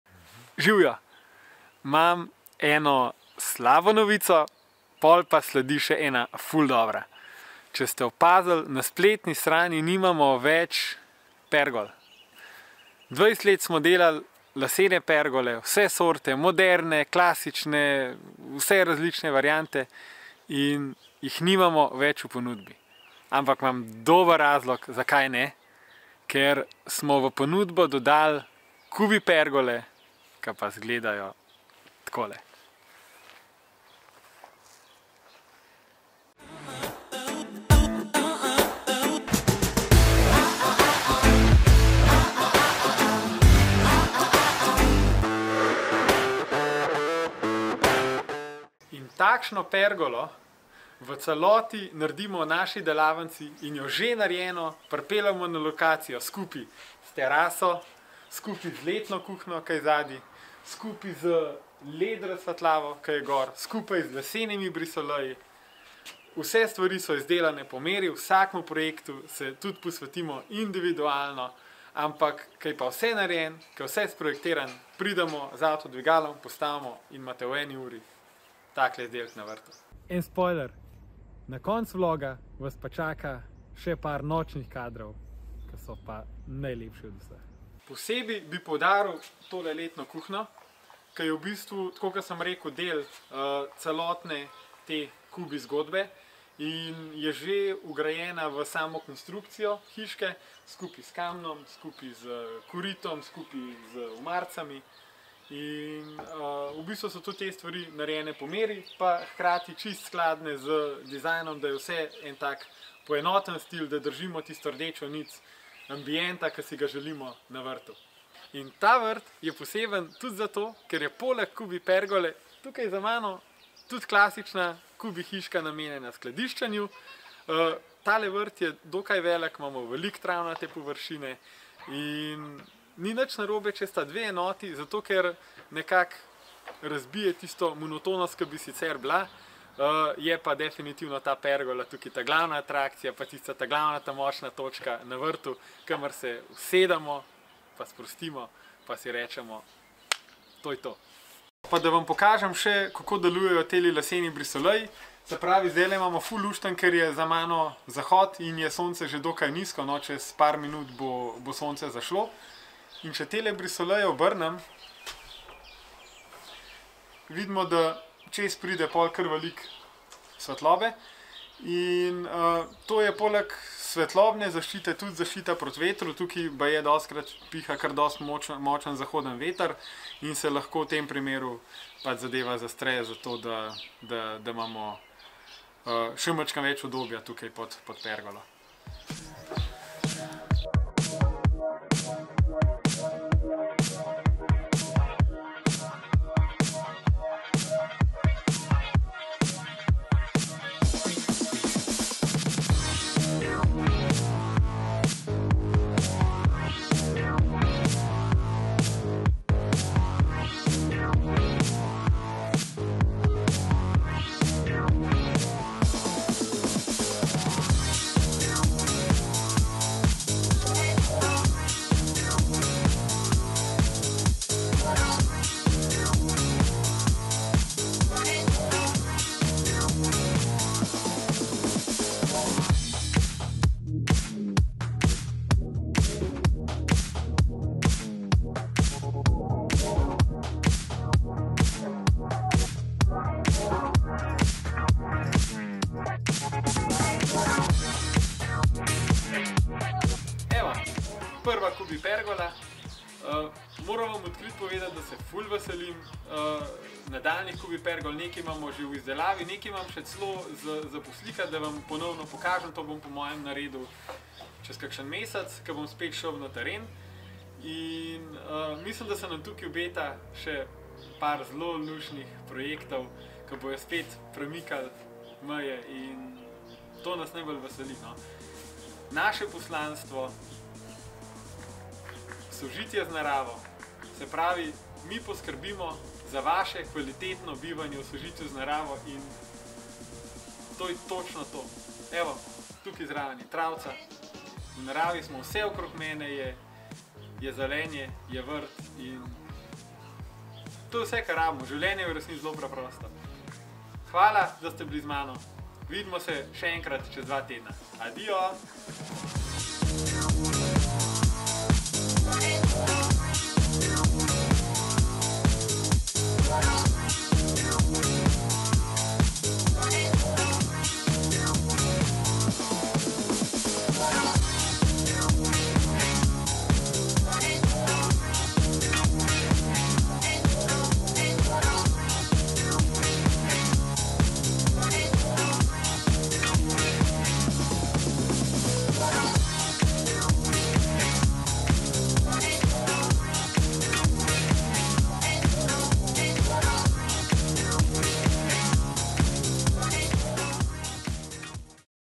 VLOG: Neverjetne pergole CUBIE se predstavijo
Jaaaaa končno so tu.

Pergole CUBIE so plod več desetletnega razvoja in gradnje pergol.

So fuzija estetike in funkcionalnosti.

A ker slika vedno pove več kot tisoč besed, si oglejte naš novi vlog, kjer jih predstavimo do potankosti.